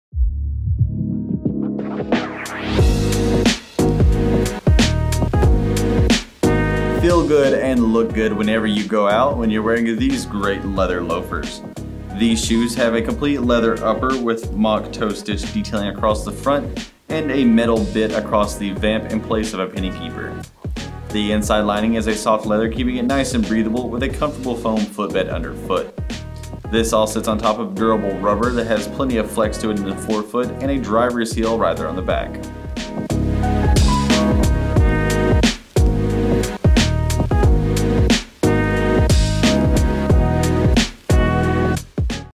BOSS Hugo Boss Driver Moccasins SKU: 9536476
Elevate your casual style with the soft, leather BOSS Hugo Boss&reg; Driver Moccasins.
Slip-on style.
The penny trim at the vamp is detailed with tonal stitching and branded hardware for signature flair.
 Set on a driver outsole for reliable grip underfoot.
Leather upper.
Leather lining and insole.
Synthetic outsole.
Made in Italy.

Product measurements were taken using size 42 (US Men's 9), width D - Medium. Please note that measurements may vary by size.